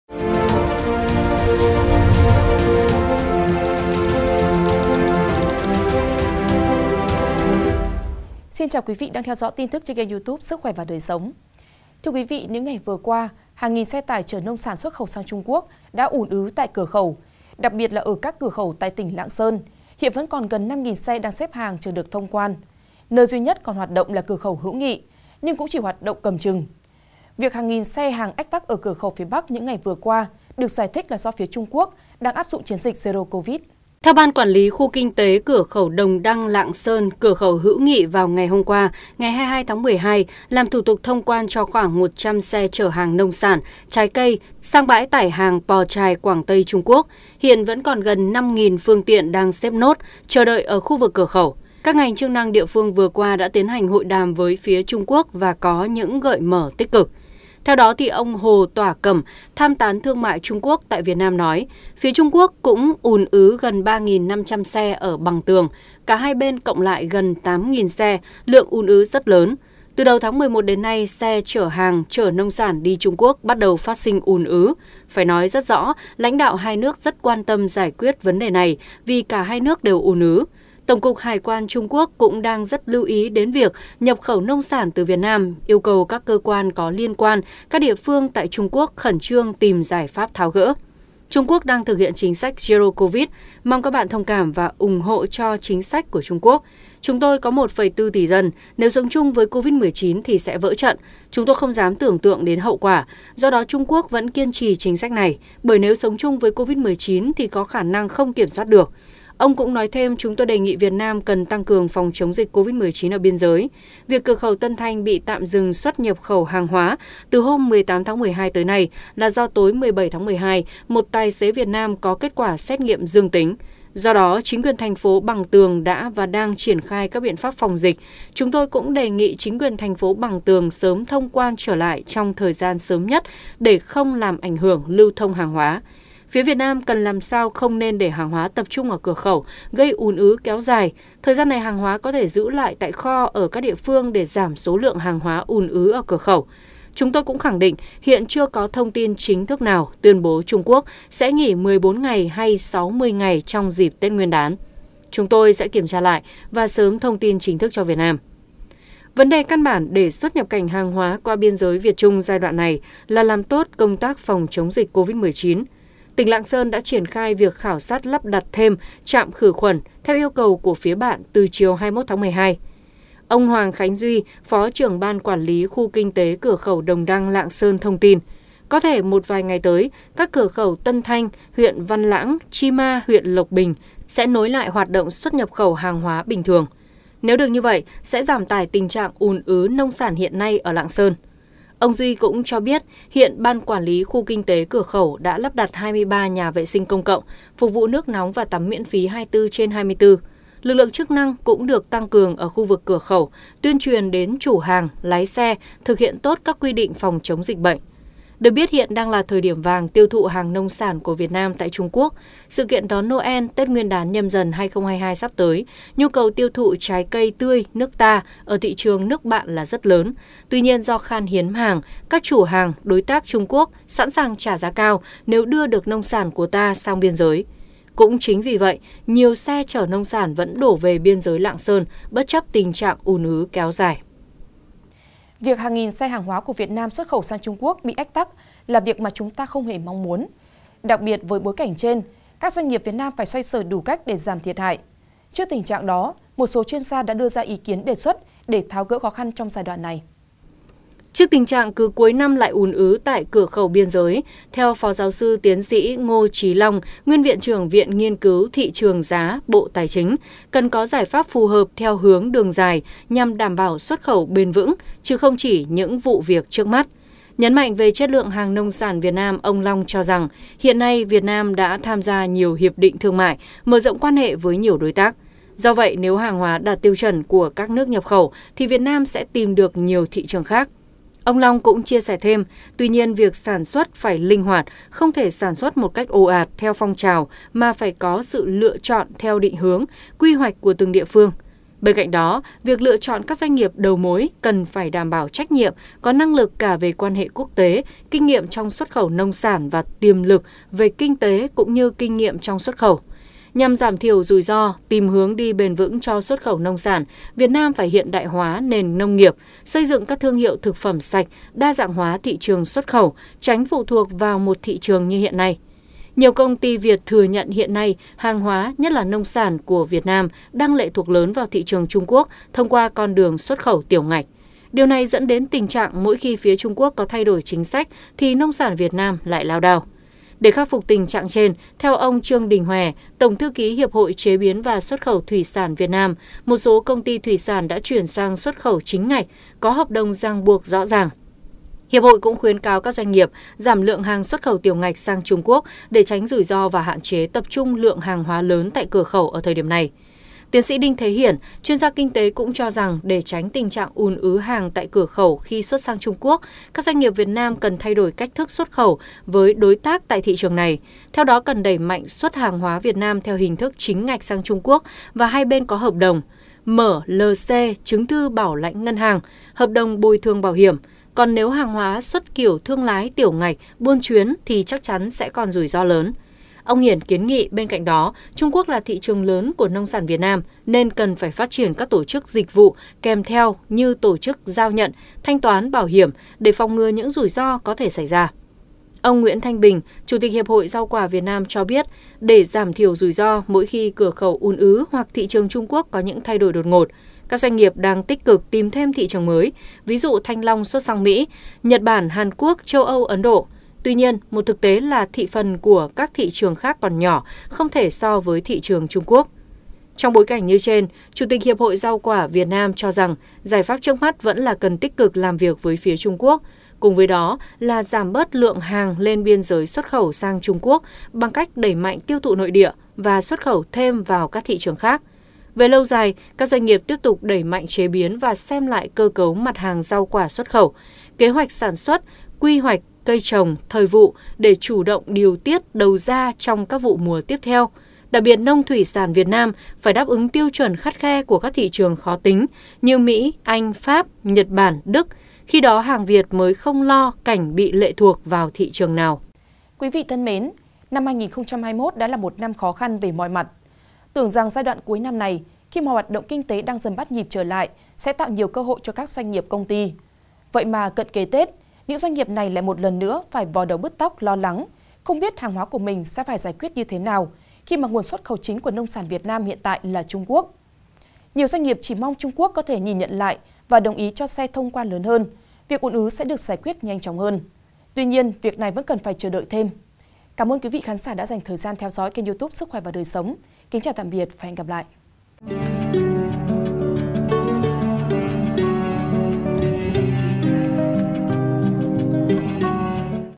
5.000 Xe Nông Sản Ùn Ứ Ở Cửa Khẩu, Trung Quốc Vẫn “Lắc Đầu” Từ Chối Vì Zero Covid | SKĐS
SKĐS | Những ngày cuối năm 2021, hàng nghìn xe tải chở nông sản xuất khẩu sang Trung Quốc đã ùn ứ tại cửa khẩu. Hiện vẫn còn gần 5.000 xe đang xếp hàng chờ được thông quan tại các cửa khẩu Lạng Sơn. Việc hàng nghìn xe hàng ách tắc ở cửa khẩu phía Bắc những ngày vừa qua được giải thích là do phía Trung Quốc đang áp dụng chiến dịch Zero Covid.

Mời quý vị và các bạn xem thêm: TP.HCM Nhận Lỗi Vì Để F0 “Bơ Vơ”; Omicron Xếp Là Chủng Dễ Lây Lan Thứ 2 Hành Tinh